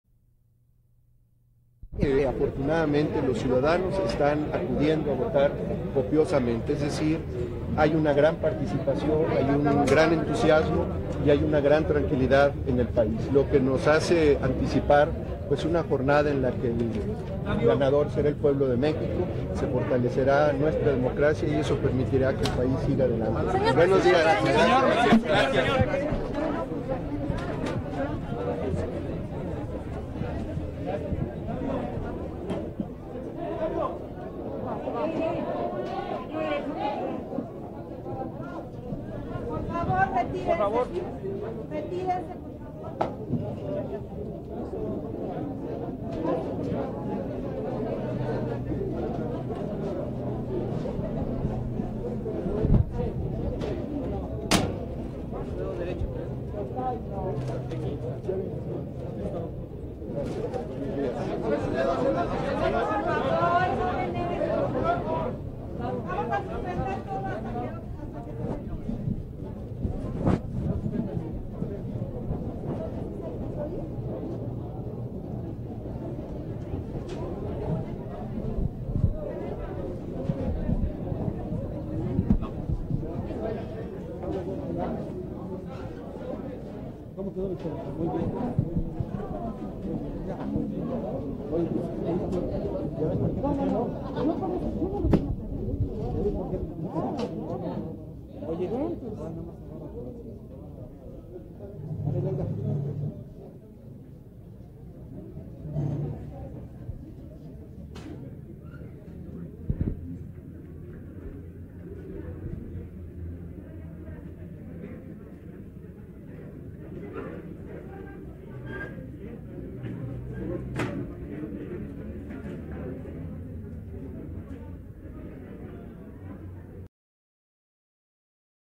Ernesto Zedillo vota en las elecciones Presidenciales del 2000
El Presidente Ernesto Zedillo acude a votar en las elecciones Presidenciales del año 2000.